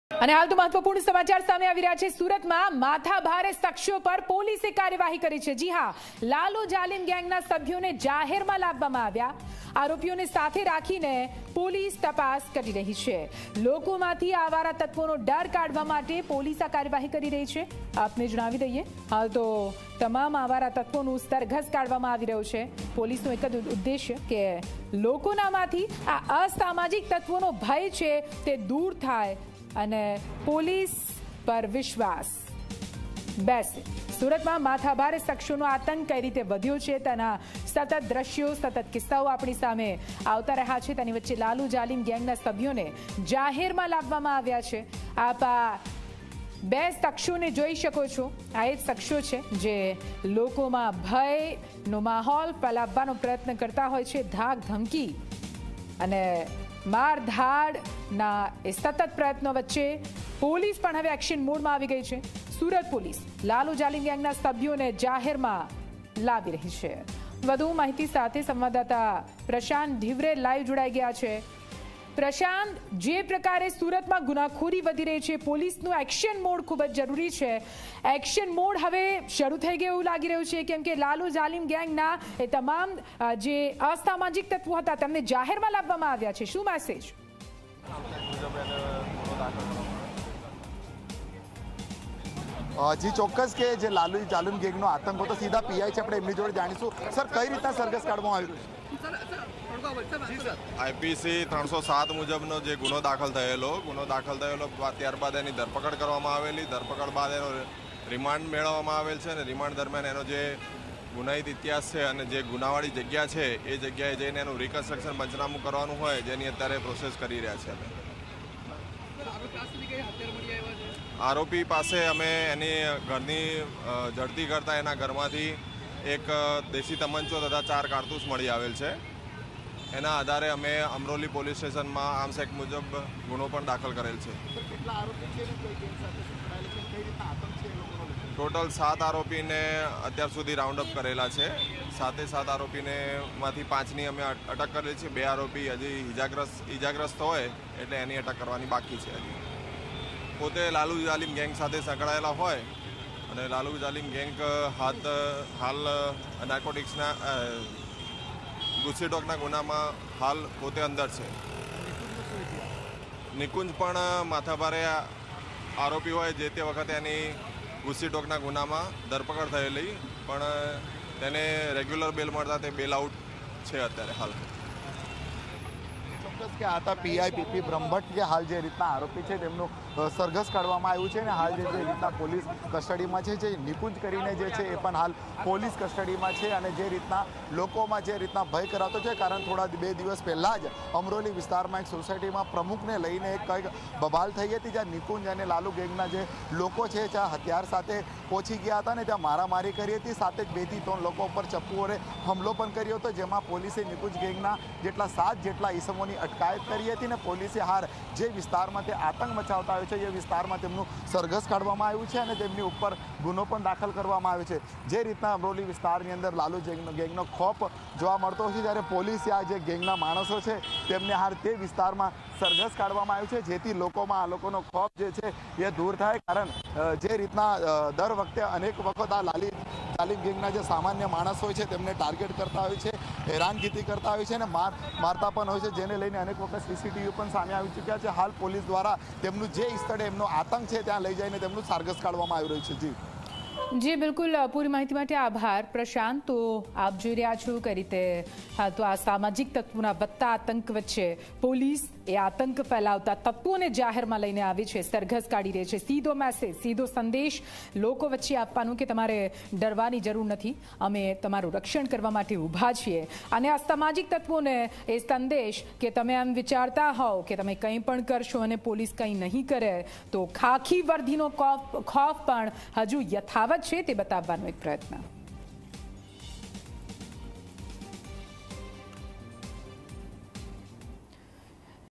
SURAT: માથાભારે શખ્સો પર પોલીસની કાર્યવાહી, લાલુ જાલીમ ગેંગના સભ્યોને જાહેરમાં લવાયા | ZEE 24 Kalak
માથાભારે લાલુ જાલીમ ગેંગના સભ્યોનું સરઘસ કાઢવામાં આવ્યું
અમરોલીમાં વિસ્તારમાં લાલુ જાલીમ ગેંગનો છે આતંક
લોકોમાંથી જાલિમ ગેંગનું  ખોફ કાઢવા નિકુંજ ચૌહાણ સહિત 5 ઇસમોનો સરઘસ કાઢવામાં આવ્યો
પોલીસે લાલુ જાલિમ અને નિકુંજ ચૌહાણ પર ગુજસીટોક ગુનો દાખલ કરાયો હતો
લાલુ જાલિમ અને નિકુંજ ચૌહાણ જેલમાં બંદ હતા
નિકુંજ ચૌહાણ ઉપર બહાર આવ્યો હતો
બે દિવસ પહેલા એક સોસાયટીના પ્રમુખ બનવા બાબતે ઝઘડો થયો હતો
લાલુ જાલીન ગેંગના માણસો જયરામ રબારીના સમર્થનમાં સોસાયટીમાં હથિયાર સાથે ઘૂસી આવ્યા હતા
લાલુ જાલીન ગેંગના માણસોએ બે લોકો પર હથિયાર વડે હુમલો કર્યો હતો
સોસાયટીમાં આતંક મચાવી ફરાર થઈ ગયા હતા
અમરોલી પોલીસે લાલુ જાલીમ ગેંગના નિકુંજ ચૌહાણ અને તેના અન્ય સાગરિકોની કરી ધરપકડ